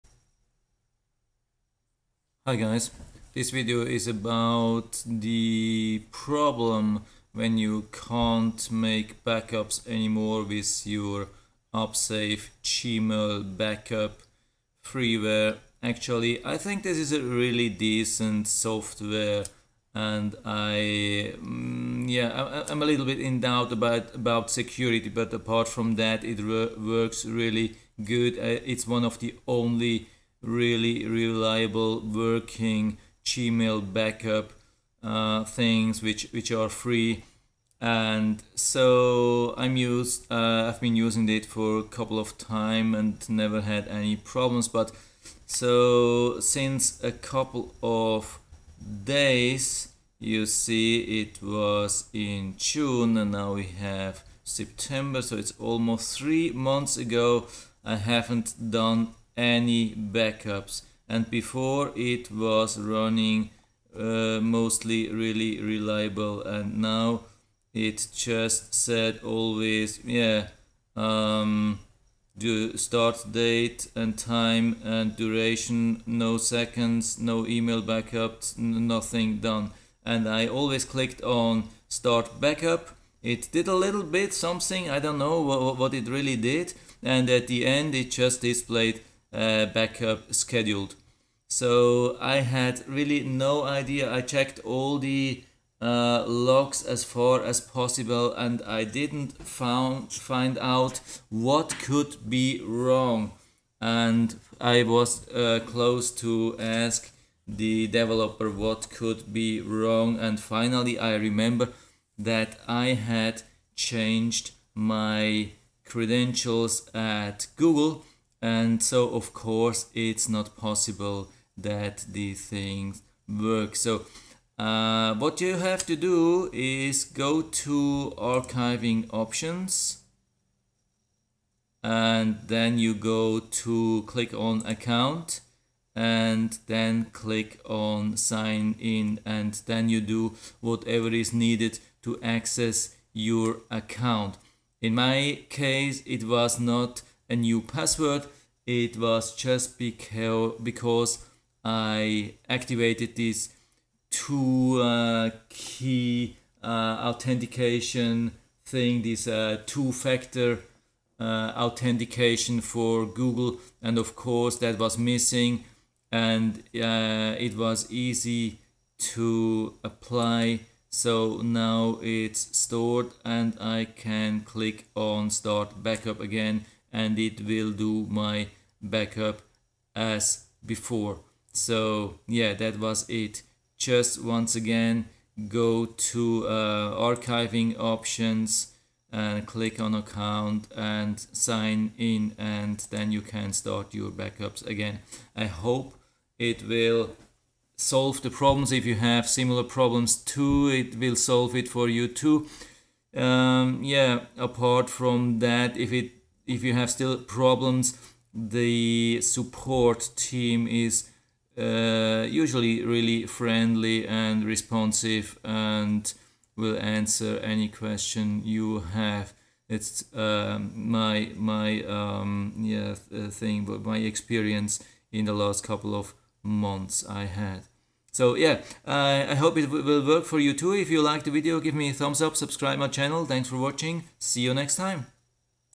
What To Do If UpSafe Gmail Backup Is NOT Running
If everything seems to be correct and the backup stops after a few seconds without a decent error message. This could help you. Watch the video!